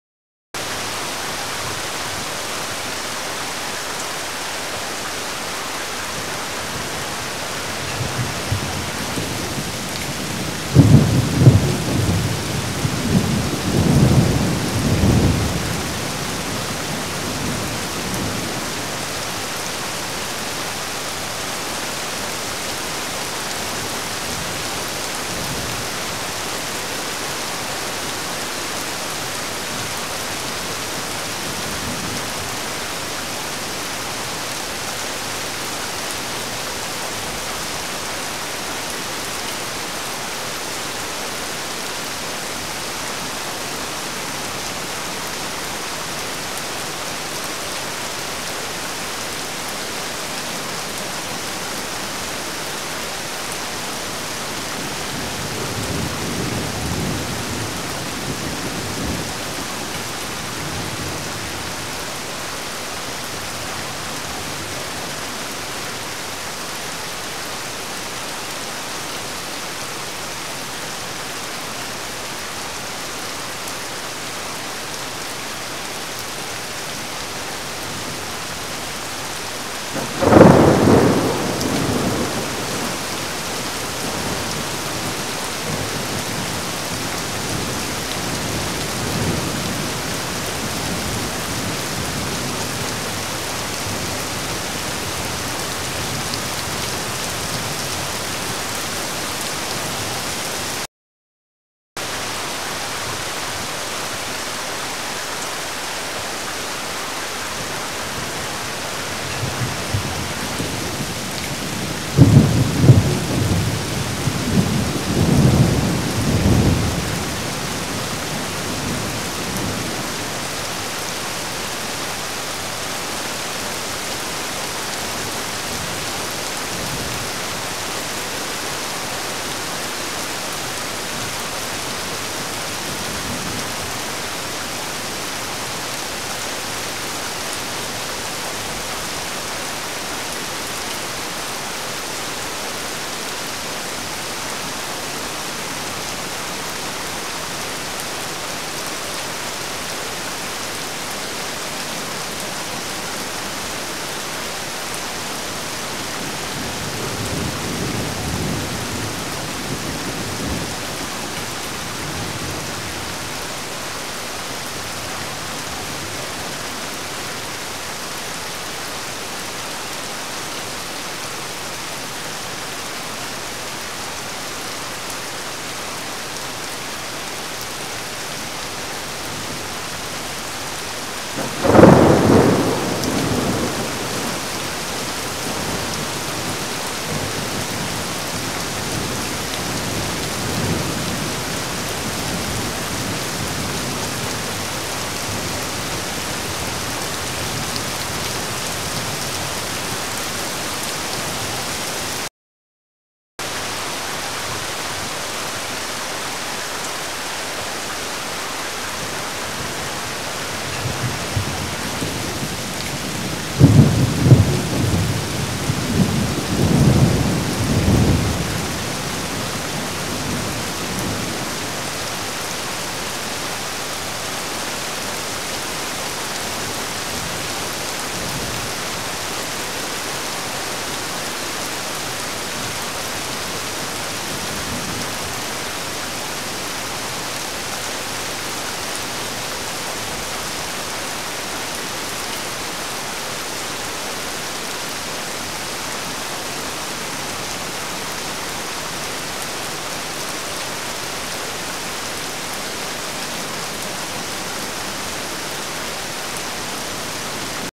Heavy Rain & Thunder To Relaxing Your Day
to get maximum results use headphones.

listen and feel ambience sounds to relax your body and mind from the daily routine.  good for insomnia or meditating.

to use this sounds in any of your videos, but you must include the following in your video description :